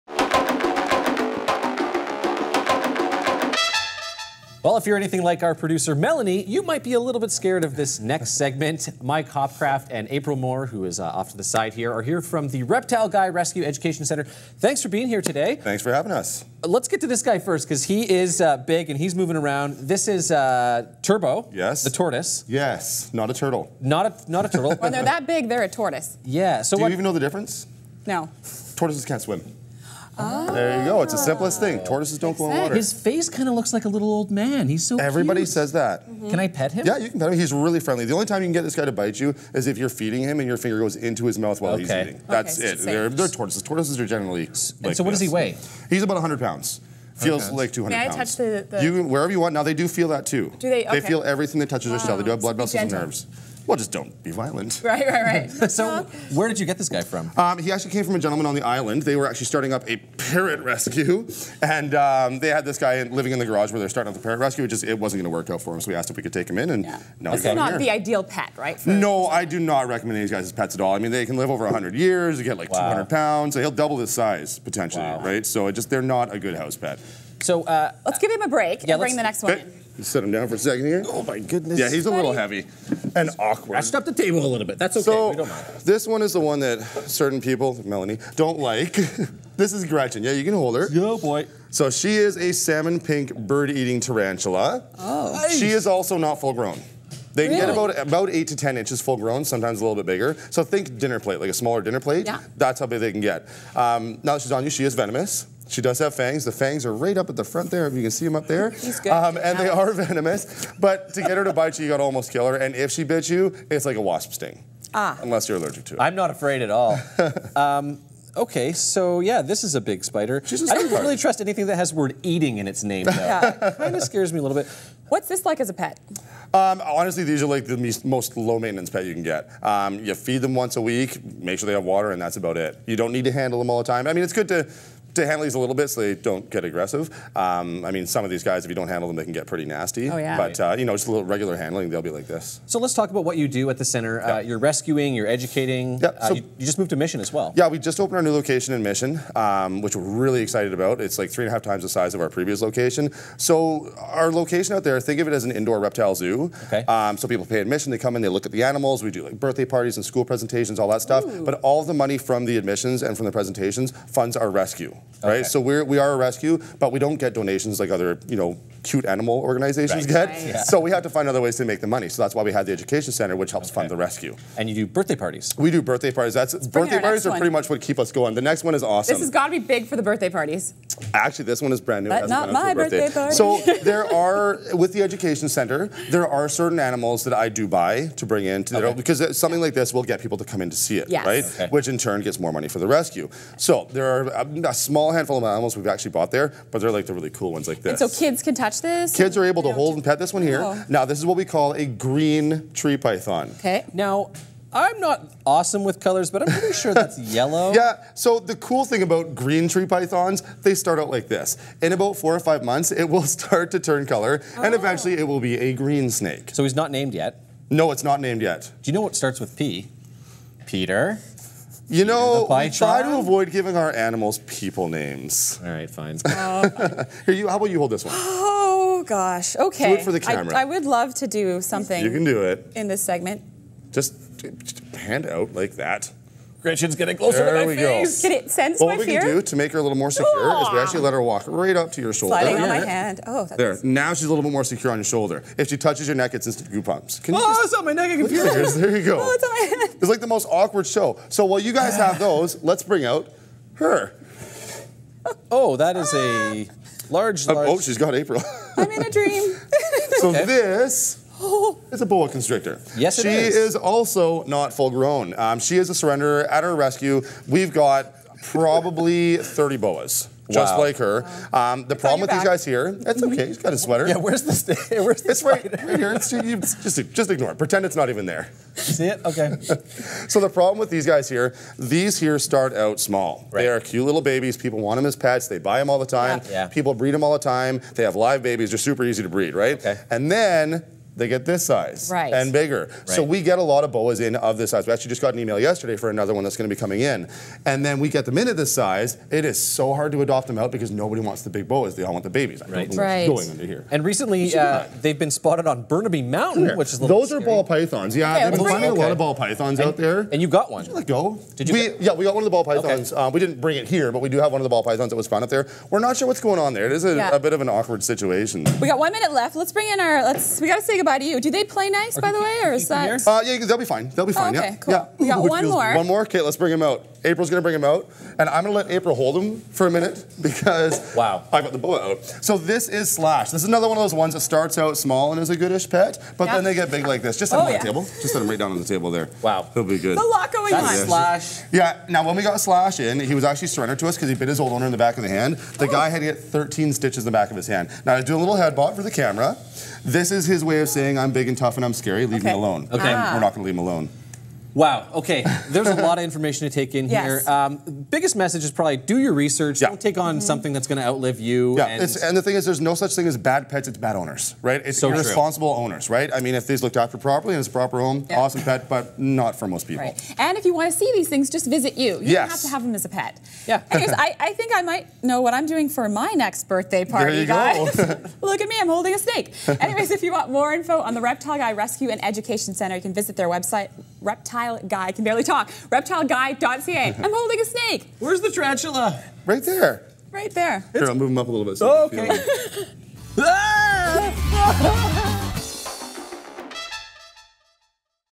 The Reptile Guy
Mike Hopcraft aka The Reptile Guy is the owner of The Reptile Guy Rescue and Education Centre in Mission, BC. The centre has over 200 reptiles and Mike stopped by with 5 of his favorites.